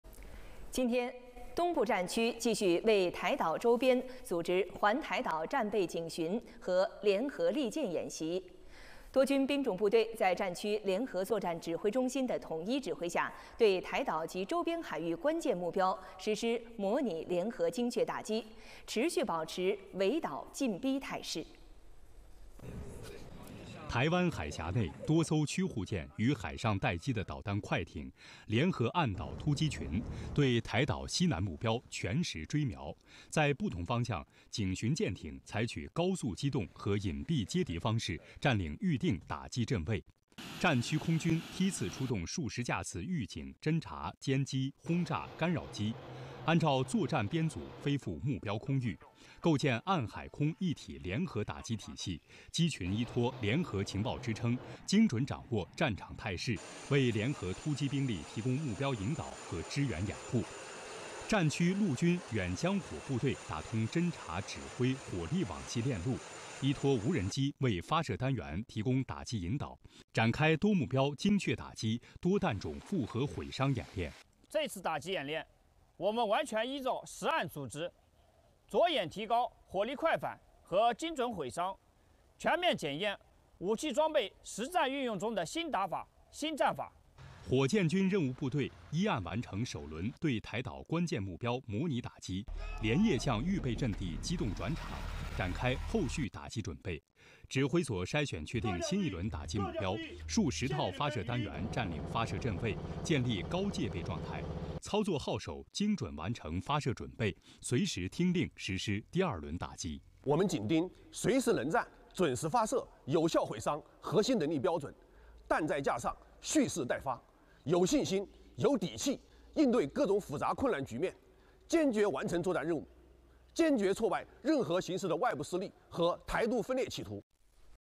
东部战区9日组织联合演练 持续保持围岛进逼态势
今天（9日），东部战区继续位台岛周边组织环台岛战备警巡和“联合利剑”演习。多军兵种部队在战区联合作战指挥中心的统一指挥下，对台岛及周边海域关键目标，实施模拟联合精确打击，持续保持围岛进逼态势。